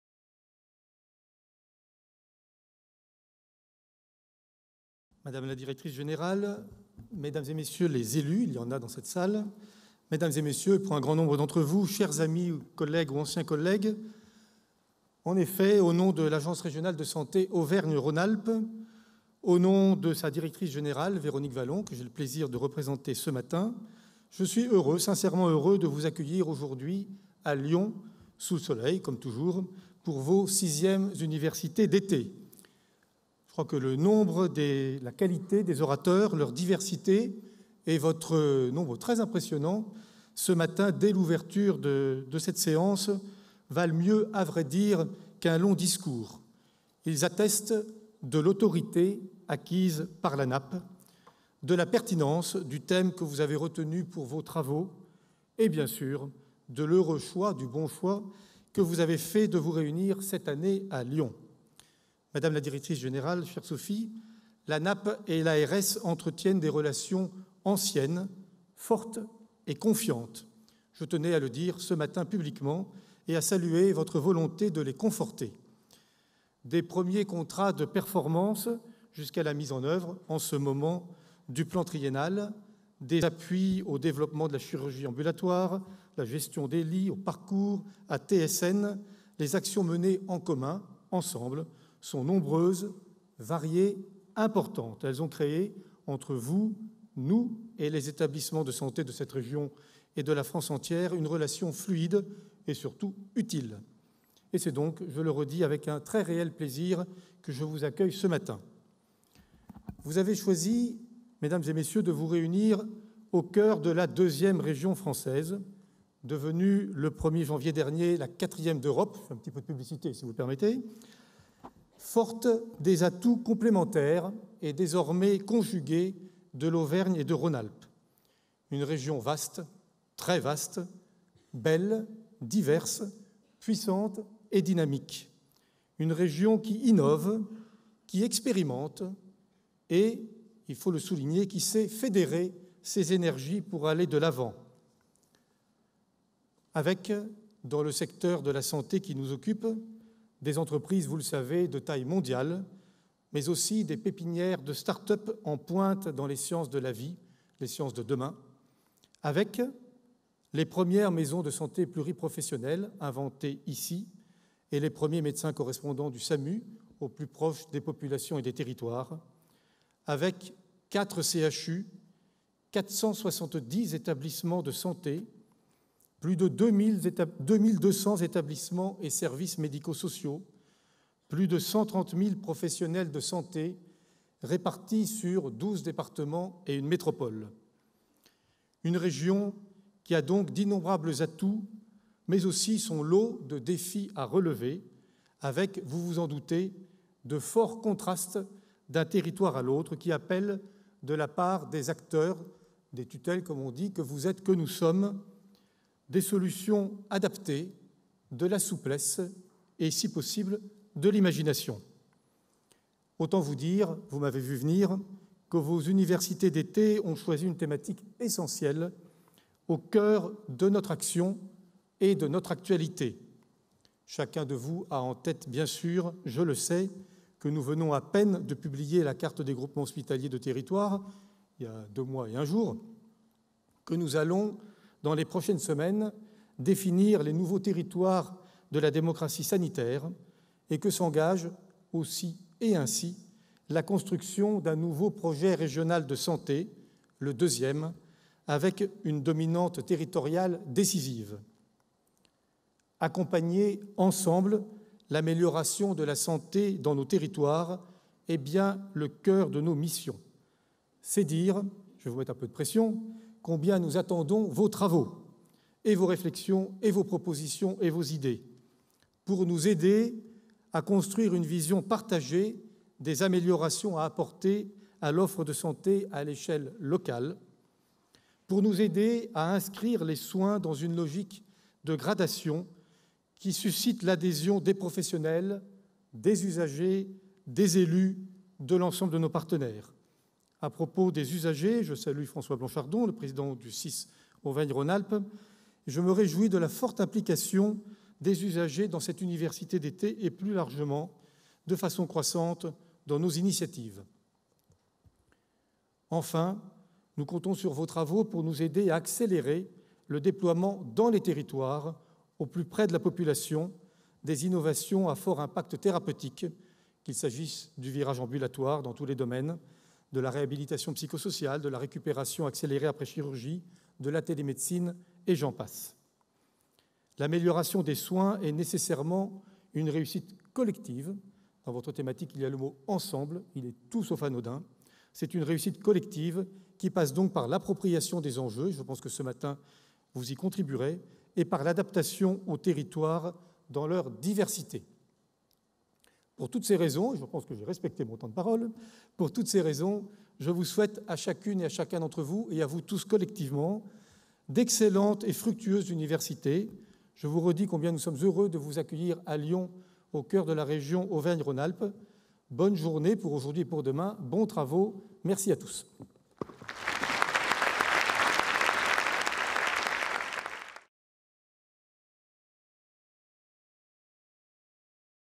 Allocution d'ouverture - Gilles de Lacaussade
Université d'été de la performance en santé de Lyon 2016 :

Allocutions d'ouverture de Gilles de LACAUSSADE, Directeur général adjoint, ARS Auvergne-Rhône-Alpes

Retrouvez les autres vidéos, les présentations des interventions, les actes et les photos de l'édition 2016